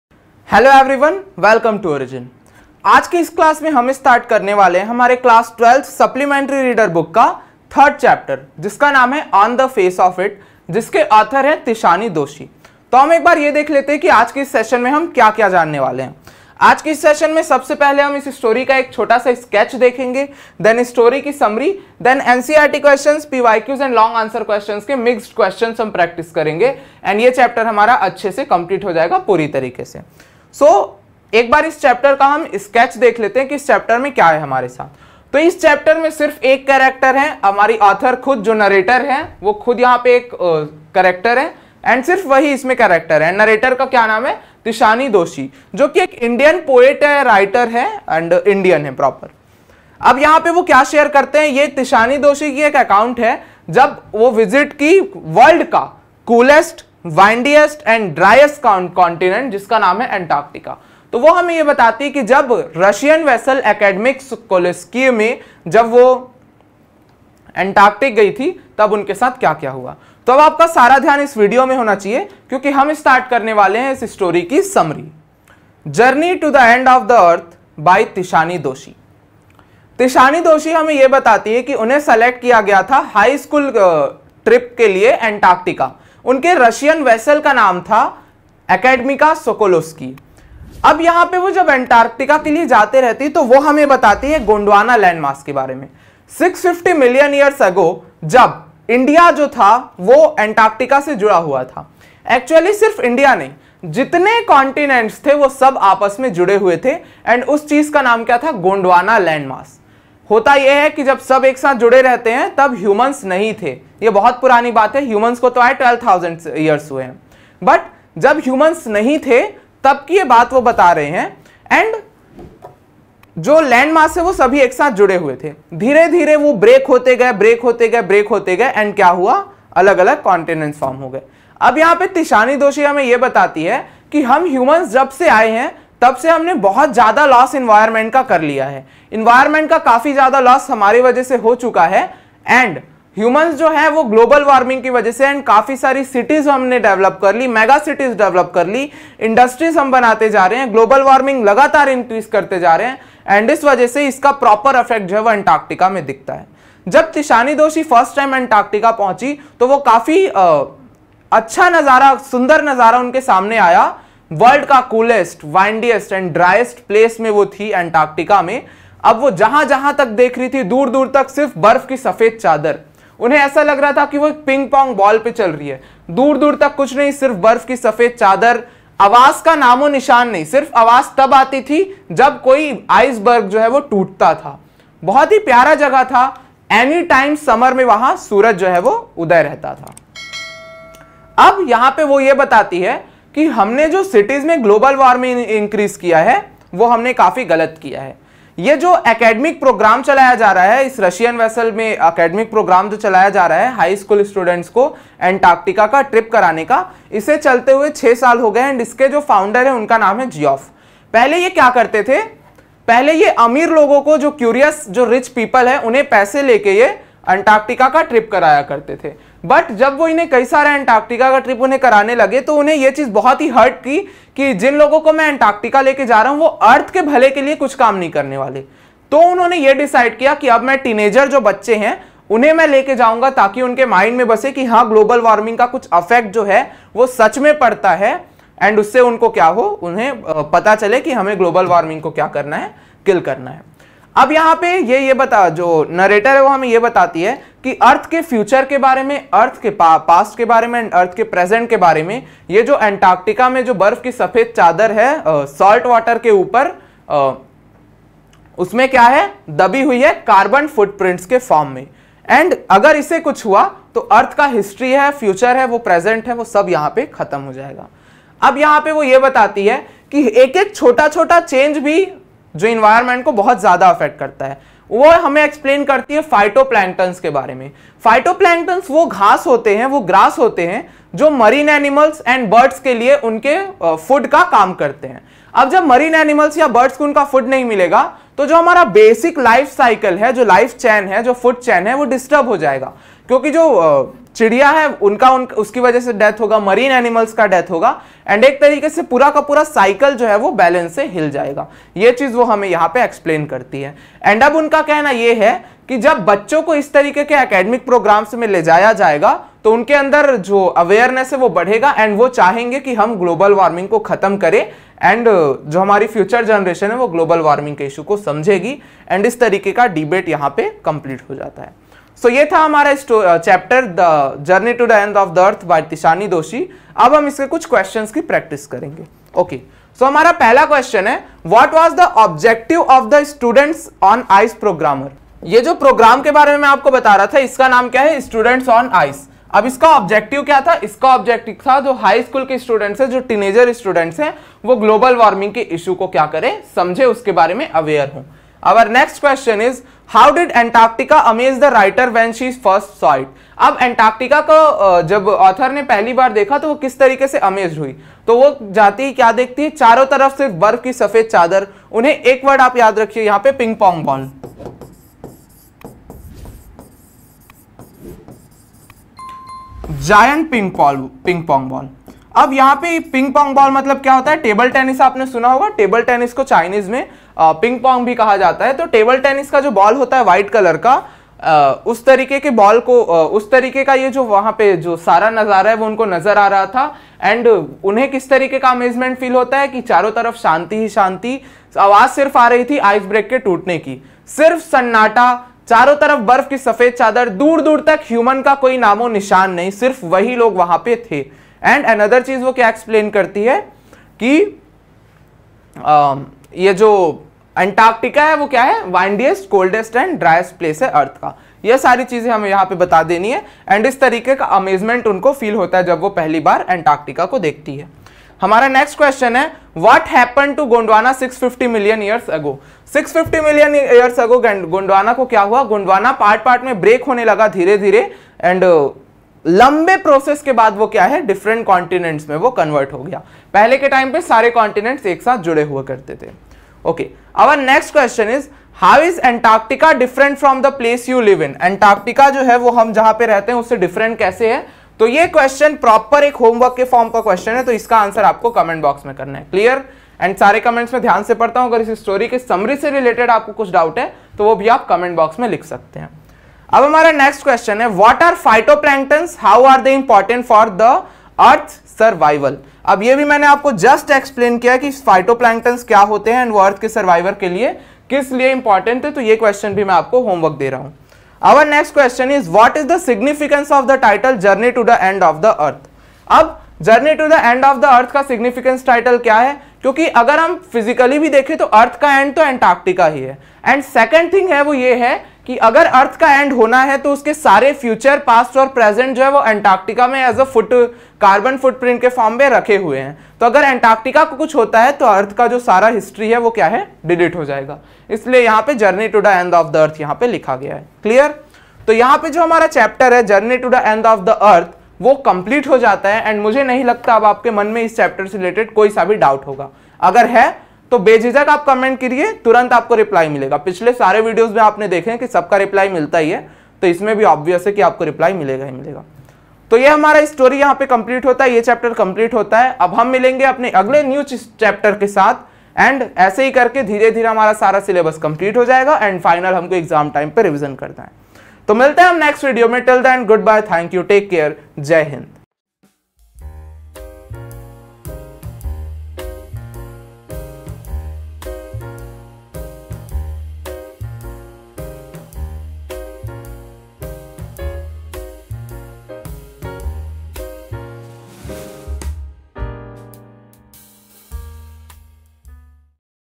Journey to the End of the Earth | English Class 12th in Hindi Chapter-3 | Summary, Ques Ans, Explain
Journey to the End of the Earth | English Class 12th in Hindi Chapter-3 | Summary, Ques Ans, Explain by @abhyuday_0108

Note : Free PDF Notes of all chapters are available in our application.